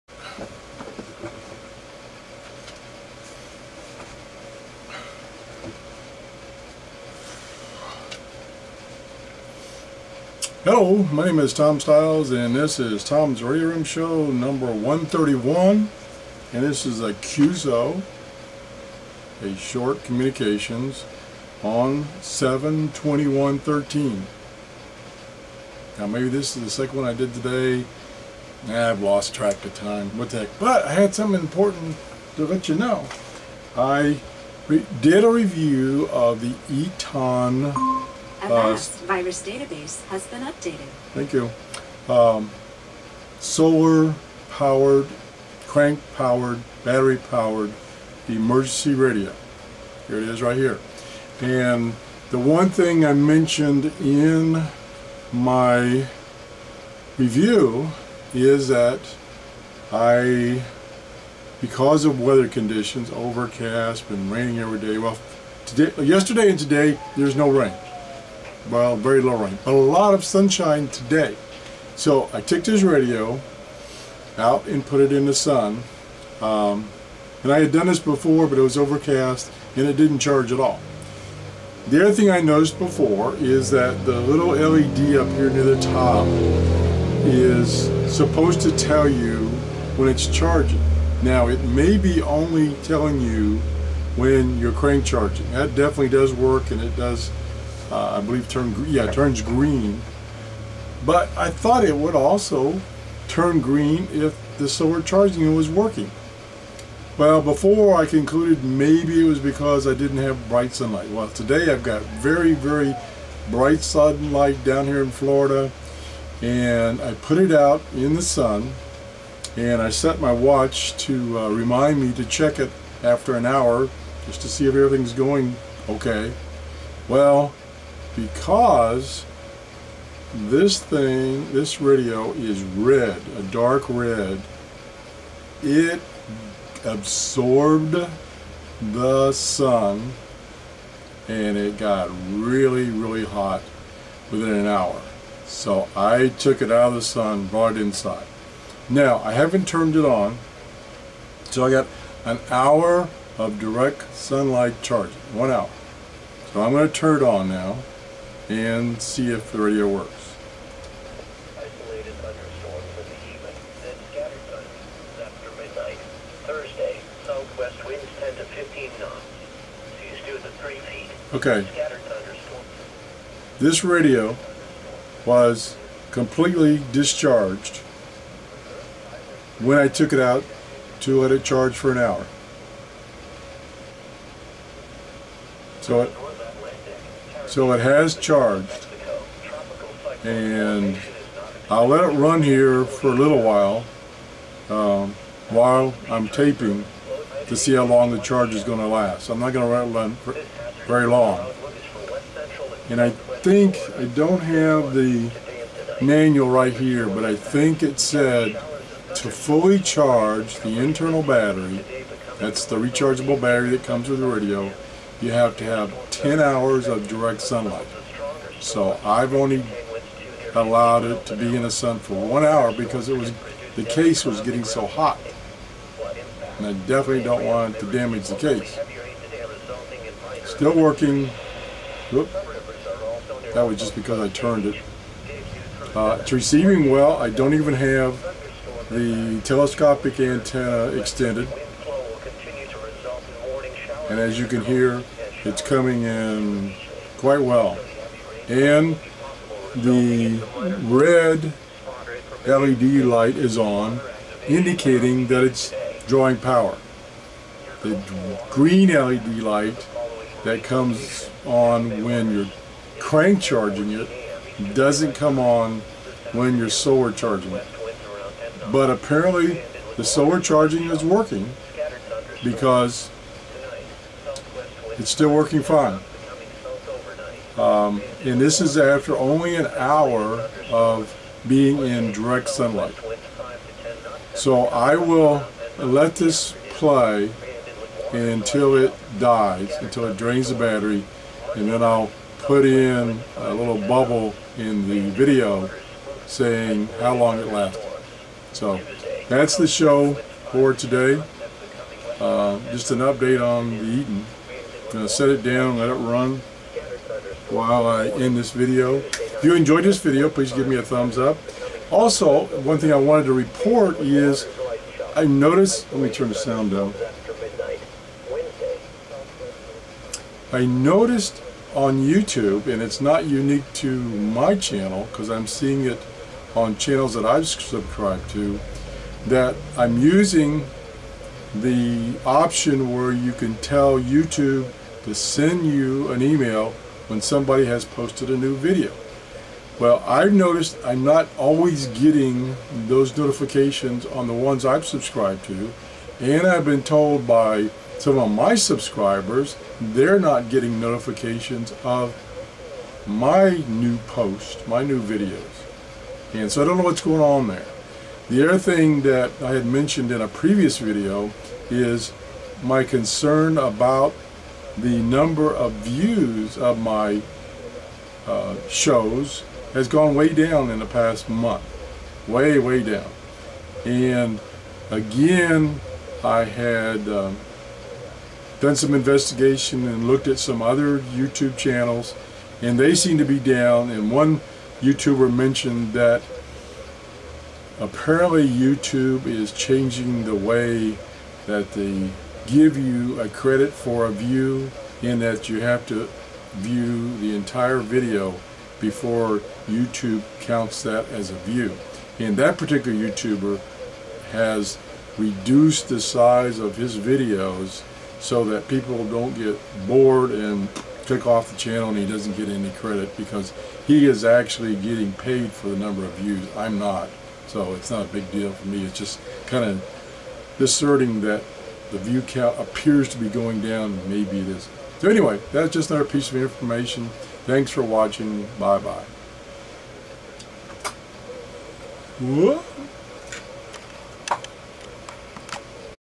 TRRS #0131 - QSO - Update on Eton Emergency Radio (solar charging)
Just a quick video on some updated testing of the Eton SolarLink FR360 emergency radio with respect to solar charging.